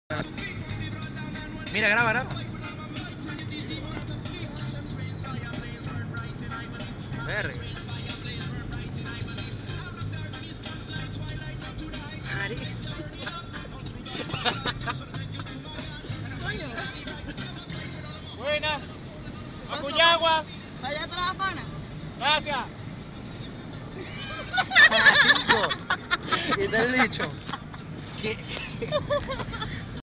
william volando a cuyagua
ocumare de la costa via cuyaguaaaaaa jajaja
protagonista: william fuenmayor @wfuenmayor4
co-protagonista: ariadna @arikcv
camarografo: eloy  @eloycalleciega
conductor: Manuel Ramirez @manuelr91